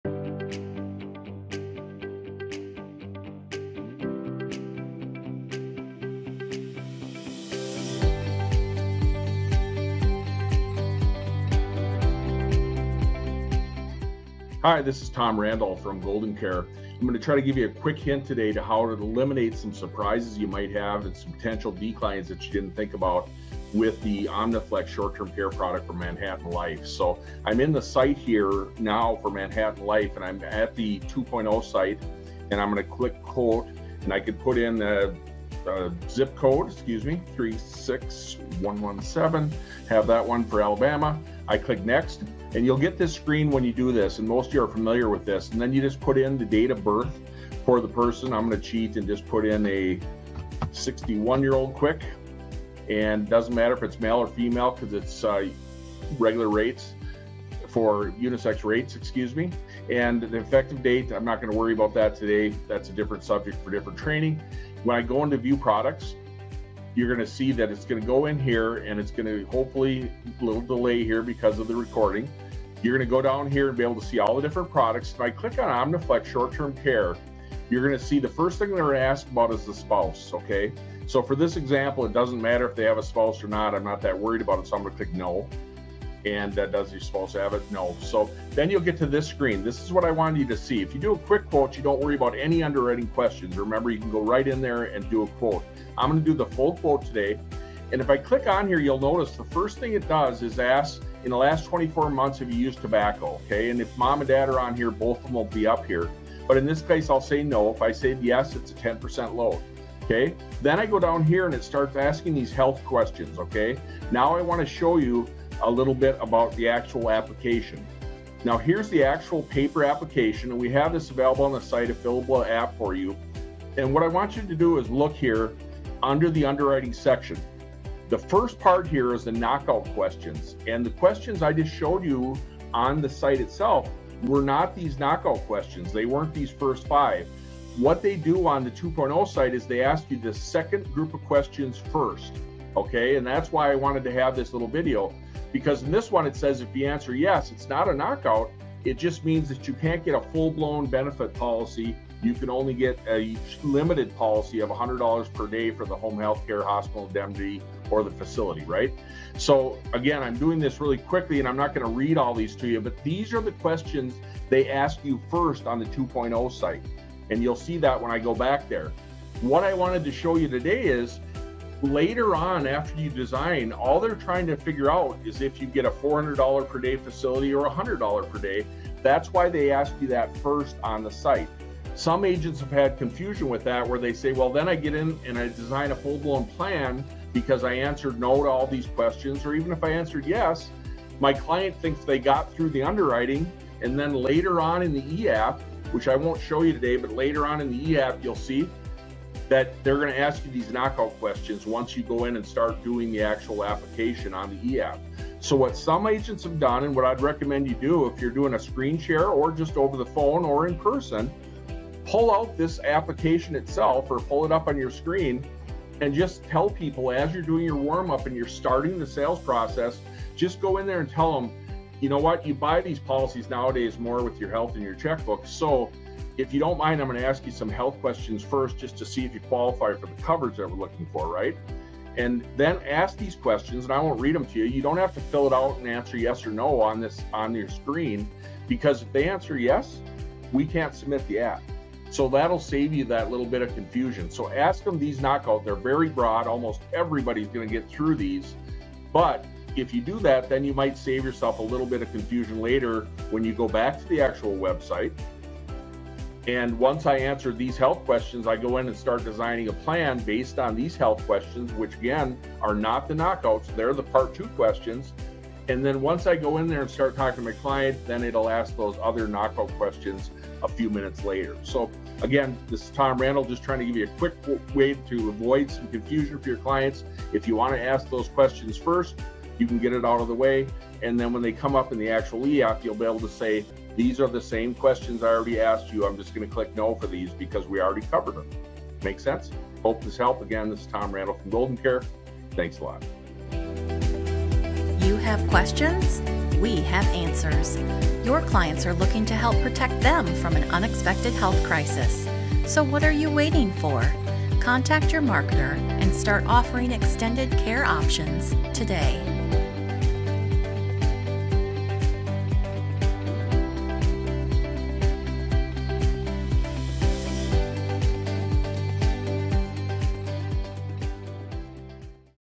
Fast Fact - OmniFlex™ - Avoid Those Declines!
OmniFlex has easy underwriting and limited knockout health questions, but make sure you avoid any decline pitfalls with these easy tips!